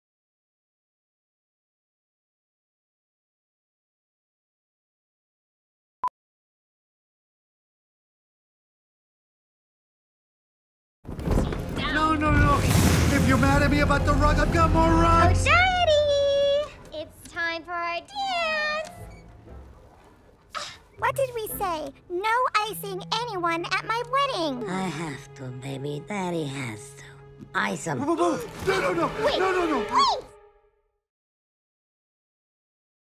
That Time I Voiced A Zootopia Character
I got to do a voiceover for the character - but just one line! It was Fru Fru, the Jersey-girl elephant shrew daughter of Mr. Big. Although I didn;t ACTUALLY voice her in the movie - that was voice artist Leah Latham - I had fun playing in the booth :) Enjoy!